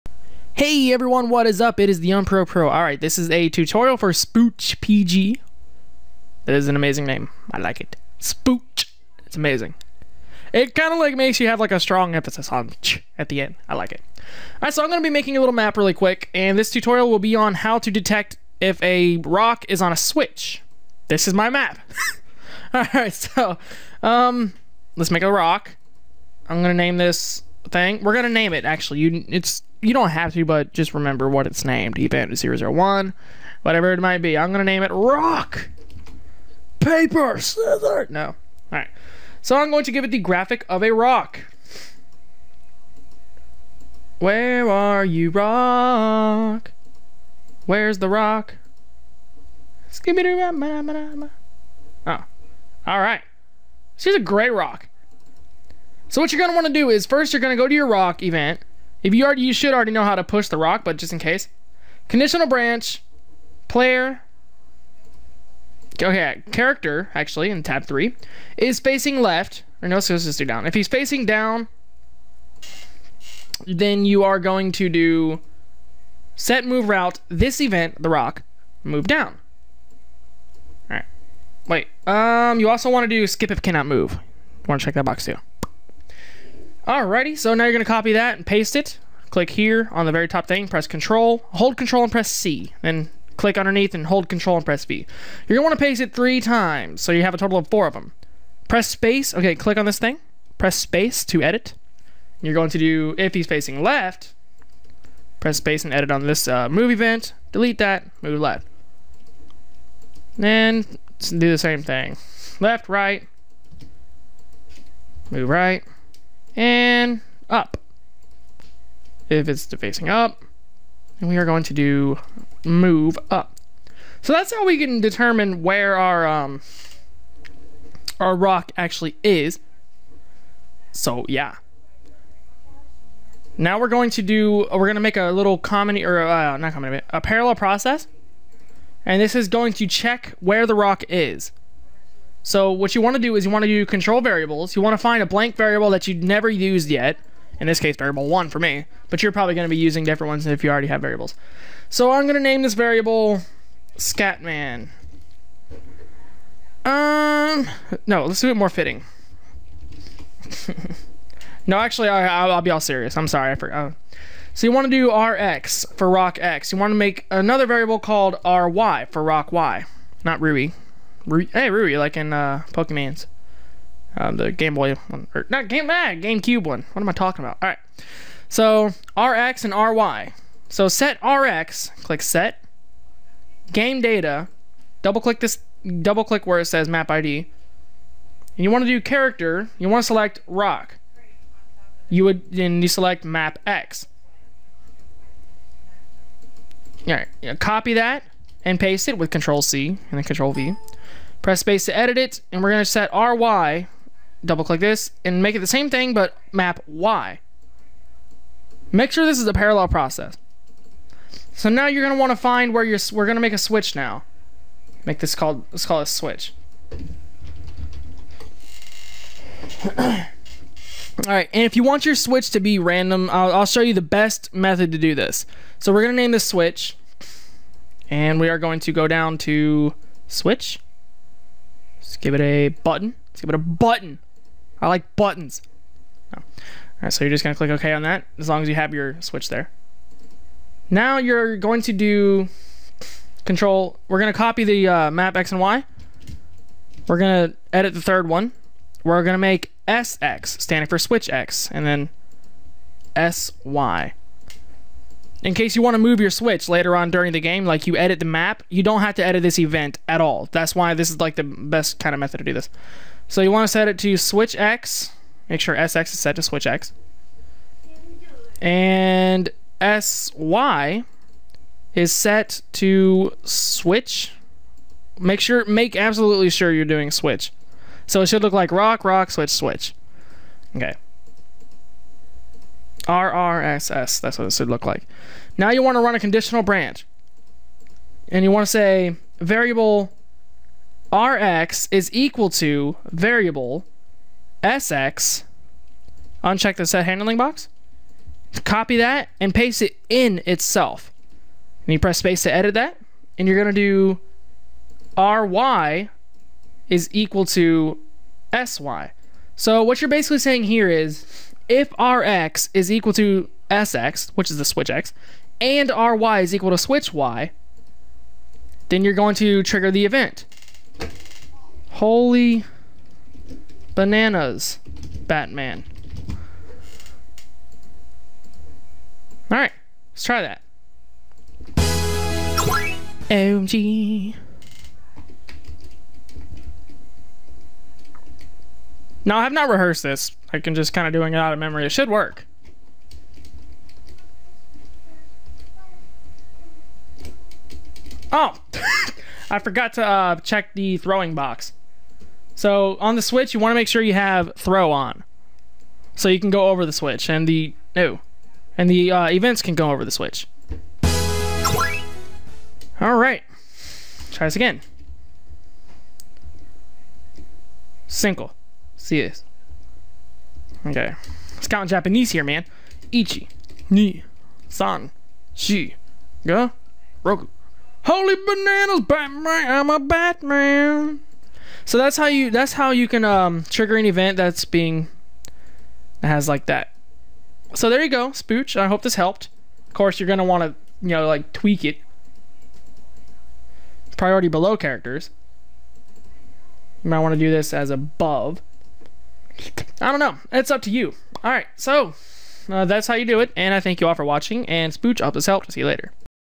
Tutorial for SpoochPG - How to make a button triggered by pushing a rock on it
This is a tutorial for SpoochPG :D How to make a button triggered by pushing a rock on it